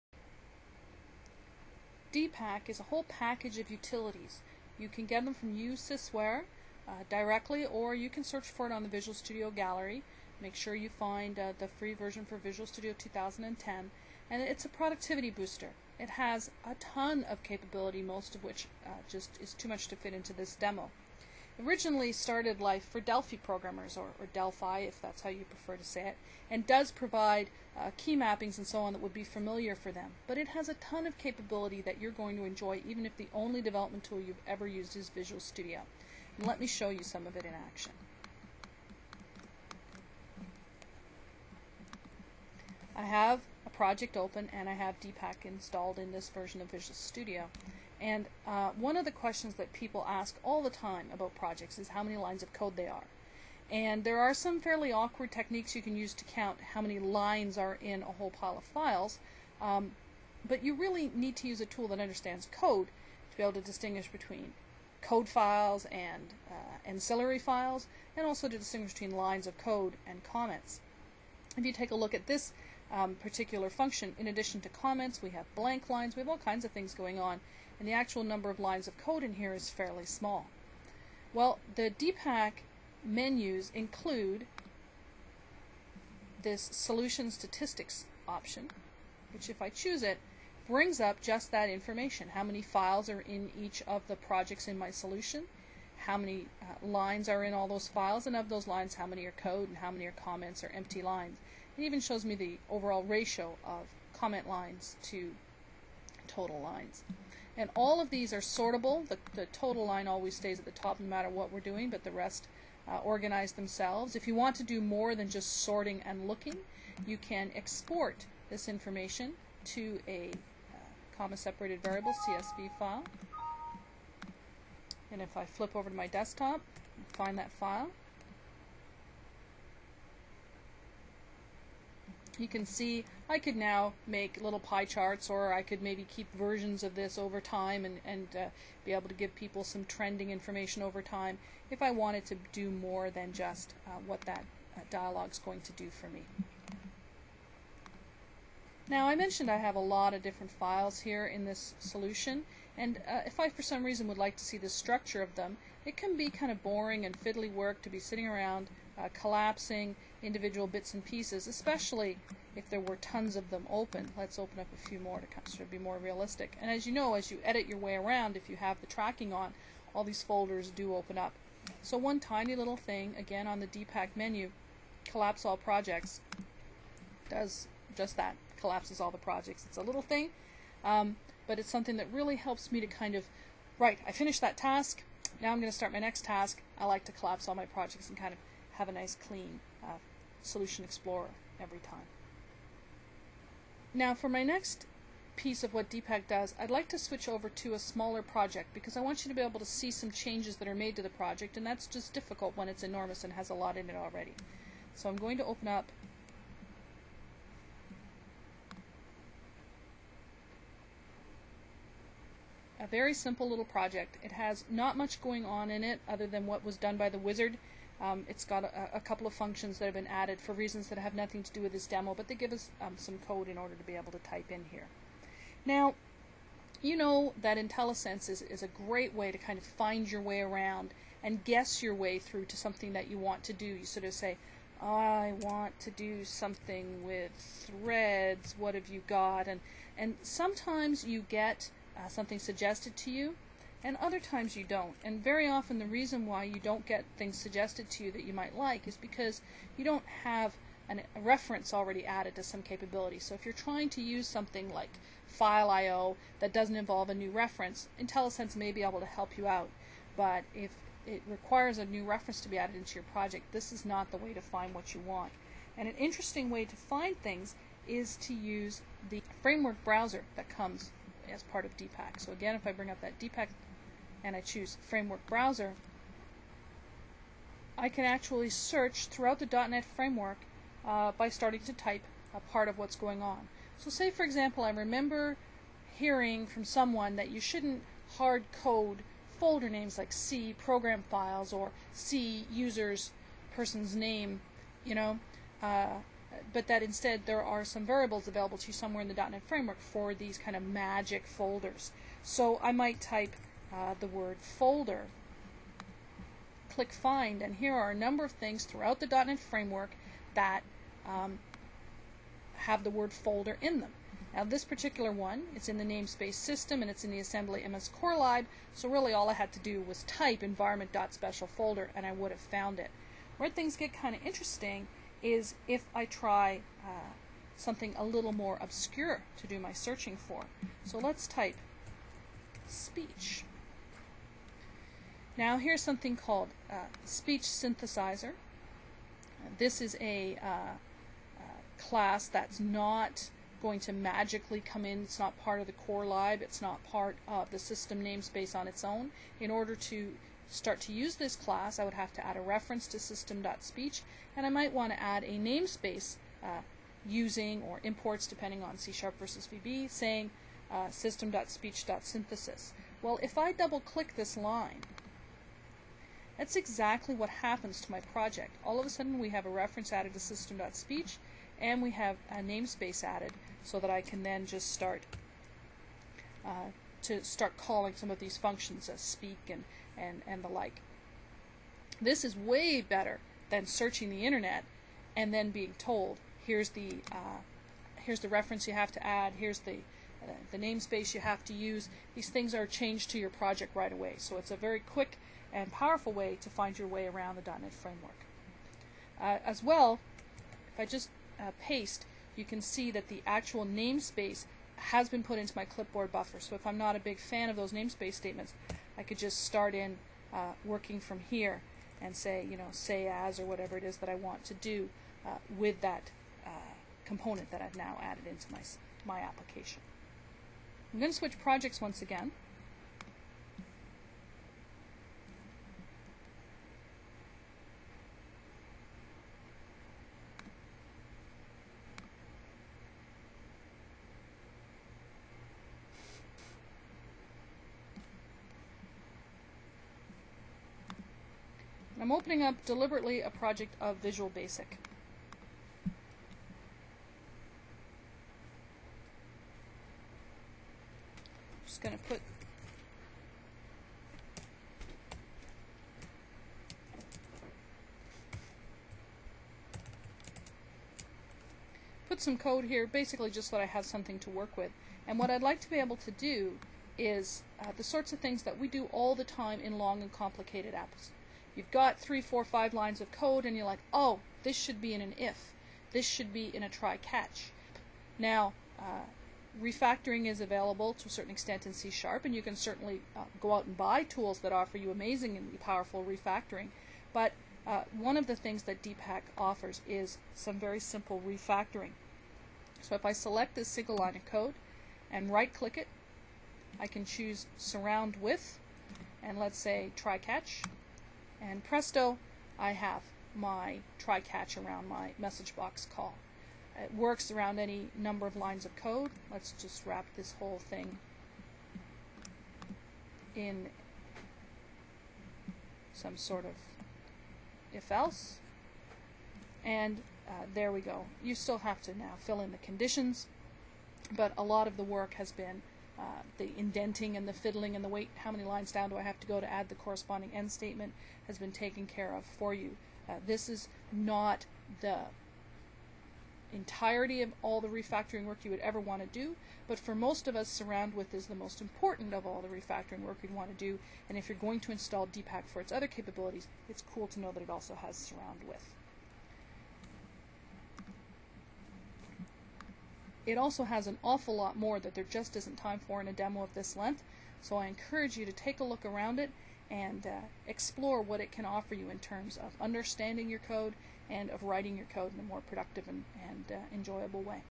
D-dp-1.wmv
Dpack demo for "Top Ten Free Extensions to Visual Studio 2010" This video is to help those preparing to present the talk.